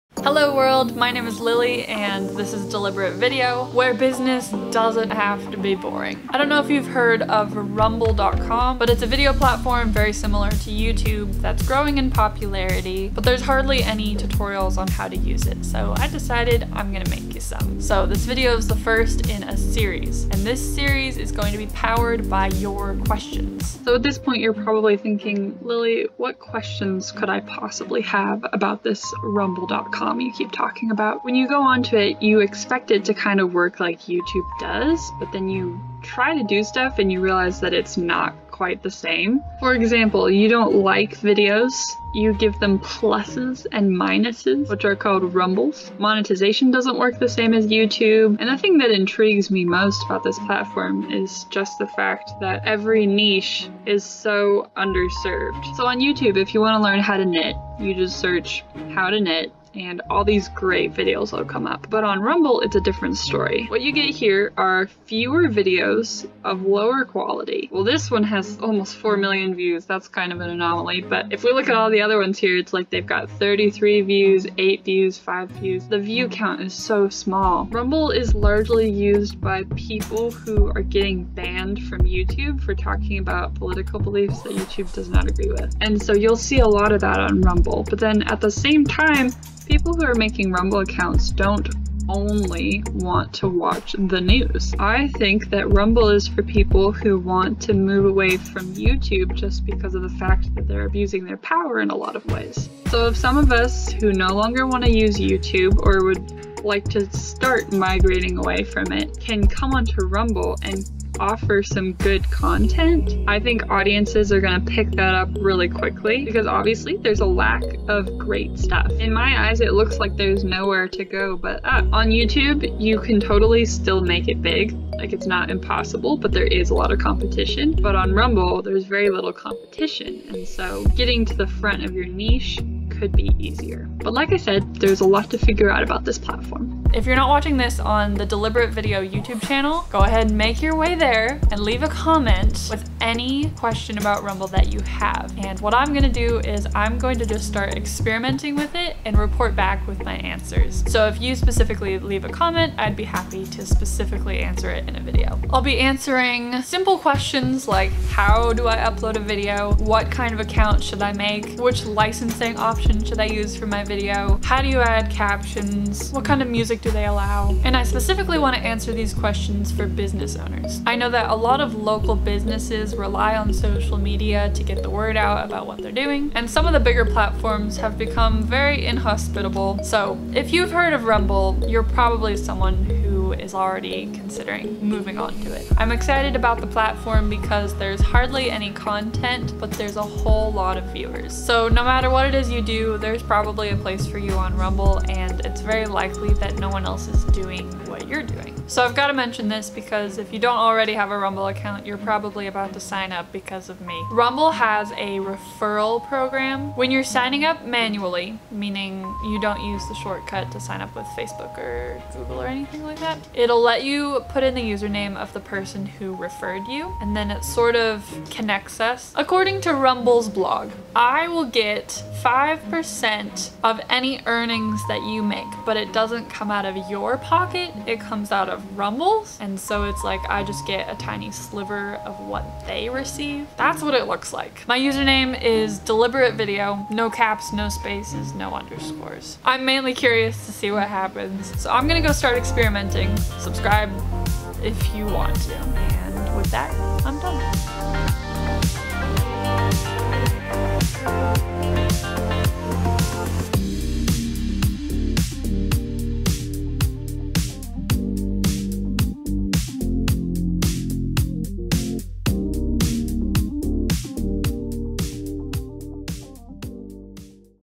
How to Use Rumble - Series Intro & Question Opportunity!
Trying to figure how to use Rumble.com for yourself or your business? Leave your questions in the comments because I'm on a mission to figure out how the platform works!

I'll be answering questions like...
Q. What kind of Rumble account should I make?
Q. Which Rumble license should I choose?
Q. How to upload a video to Rumble
Q. How to add captions to a Rumble Video
Q. What kind of music can I use in rumble videos
Q. How to make money on Rumble
Q. How to use the Rumble Referral Program

We are an entertainment based strategic video agency located in Coeur d'Alene, Idaho. Go to deliberatevideo.com to get in contact with Lilly!